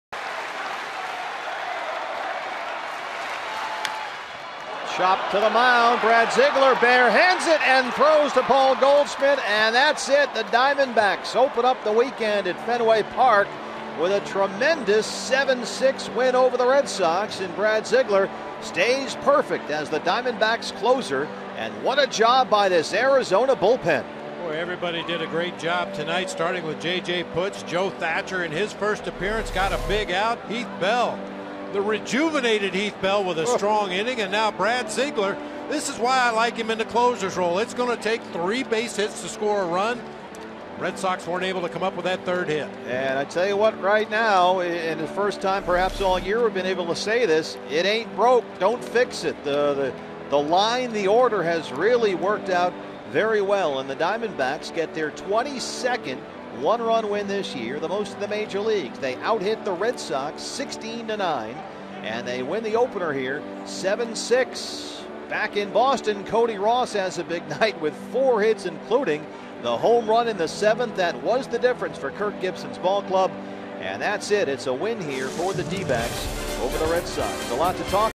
ARI@BOS: Ziegler retires Pedroia to seal the win
8/2/13: Brad Ziegler gets Dustin Pedroia to ground out back to the mound, earning his sixth save of the season in a 7-6 D-backs win